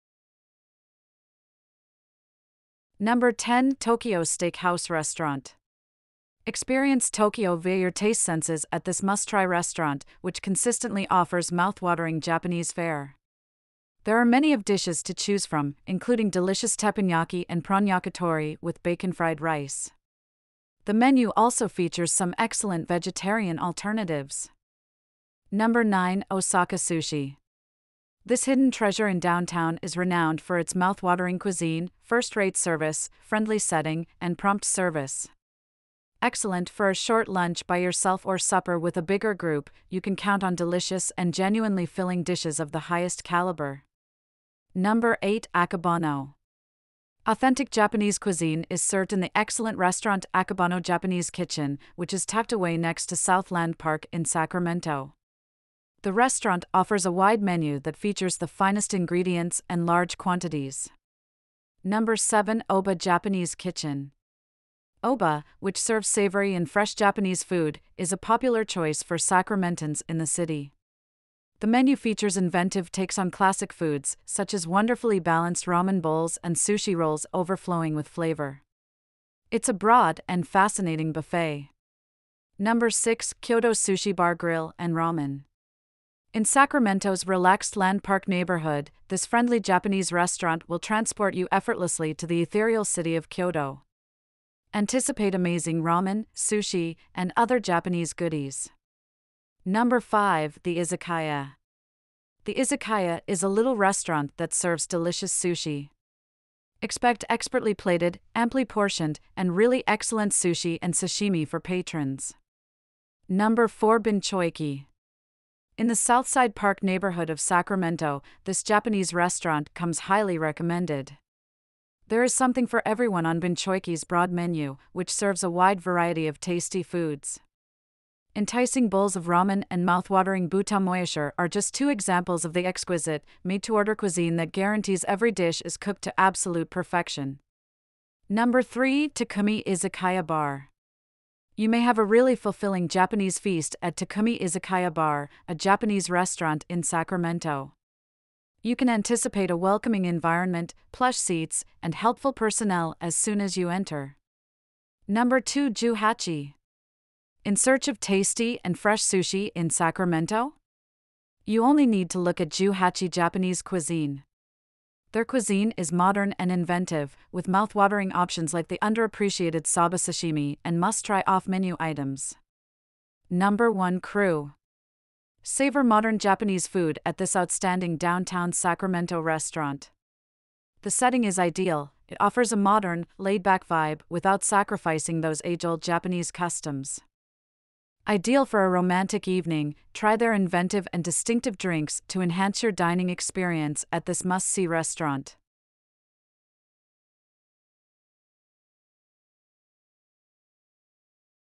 Top 10 Best Japanese Restaurants in Sacramento, California | USA - English
We spent a long time in finding a list of Top 10 Best Japanese Restaurants to Visit in Sacramento. It includes all of the different entertainment options including things to do in Sacramento. There are hundreds of other locations here, but we have selected the famous Japanese Restaurants to Visit in Sacramento for you, so you can enjoy your visit. There are many beautiful Japanese Restaurants in Sacramento which people prefer, it depends on their own interest.